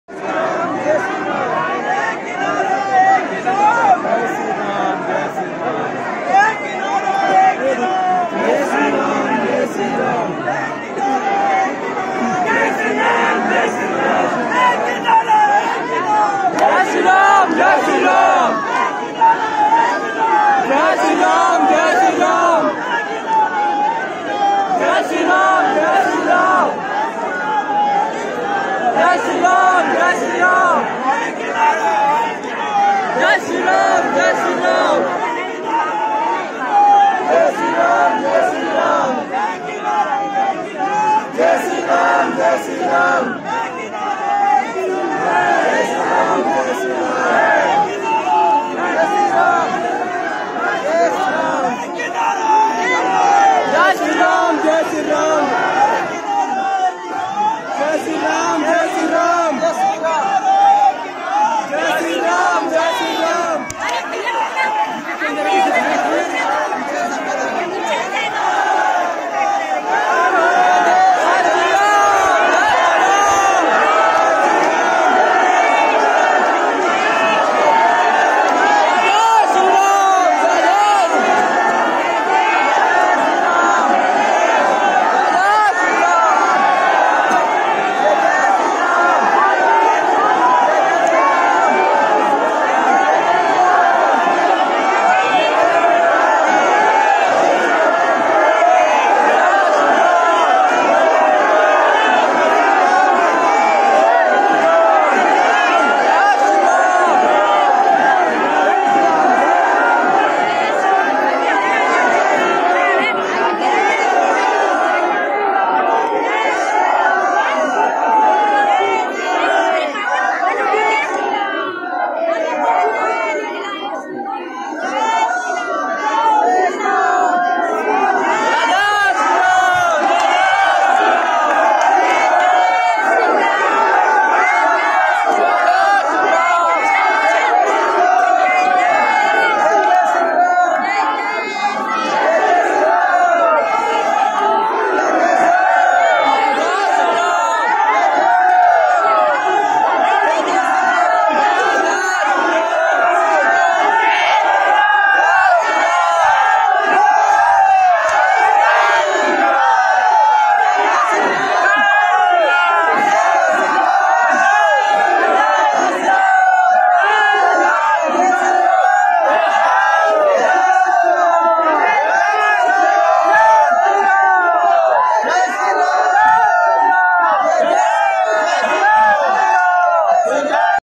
आचार्य श्री धीरेंद्र कृष्ण शास्त्री जी का दर्शन | बागेश्वर धाम सरकार के लिए हजारों भक्तों की भीड़ मे
बागेश्वर बालाजी पीठाधीश्वर आचार्य श्री धीरेंद्र कृष्ण शास्त्री जी की दर्शन करने के लिए हजारों लोगों की भीड़ मे दर्शन प्राप्त हुए जिनकी महिमा विदेशों तक फैला हुआ है

जय बागेश्वर बालाजी
जय बागेश्वर धाम सरकार
जय सन्यासी बाबा
जय अचार्य श्री धीरेंद्र कृष्ण शास्त्री